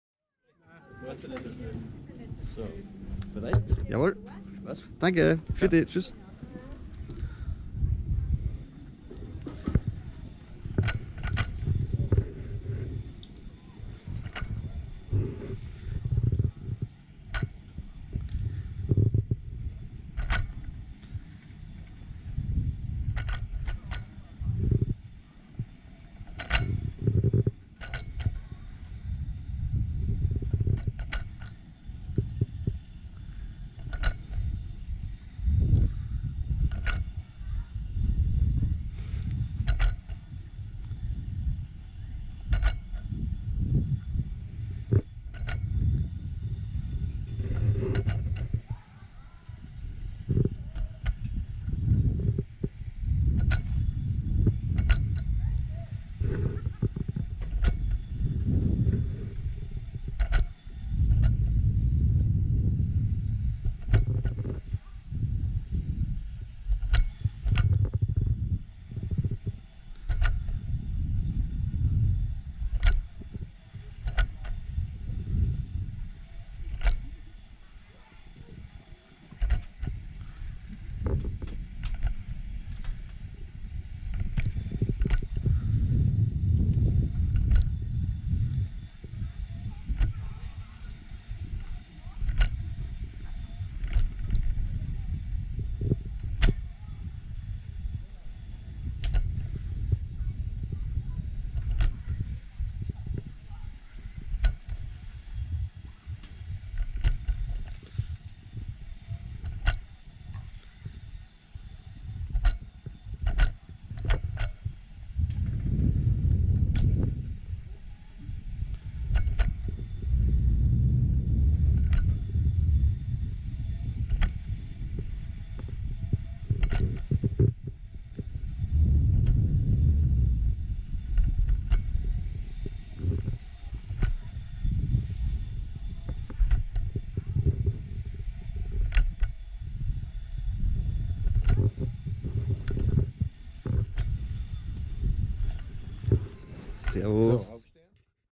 Fly-Line Rollercoaster Zipline WALD8erBAHN Flylinas Hexenflug Bürgeralpe Mariazell 2021 POV Onride
Länge bergab / length downhill 350m